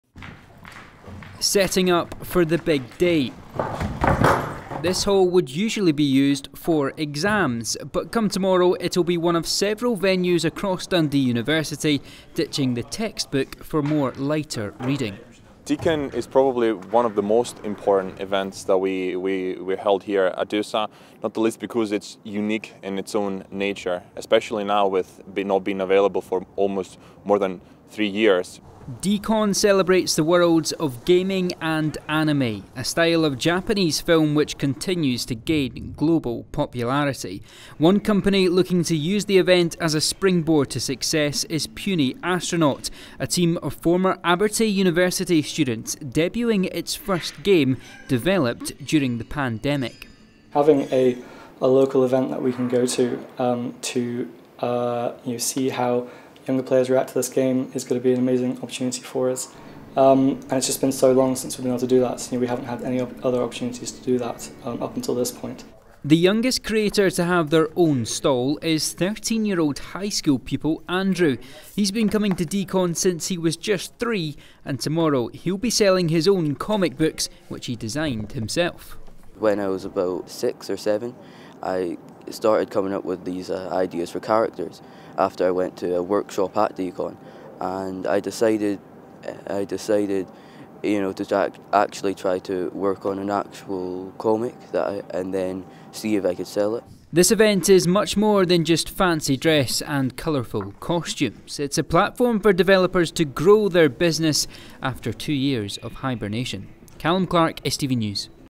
Return of Dundee anime and gaming convention 'amazing opportunity' for local developers
The return of Dundee's largest anime and gaming convention is being described as an 'amazing opportunity' for local developers.

More than 2000 people are expected to attend DeeCON this weekend - the first time it's been held since the pandemic started.

Our reporter Callum Clark has more.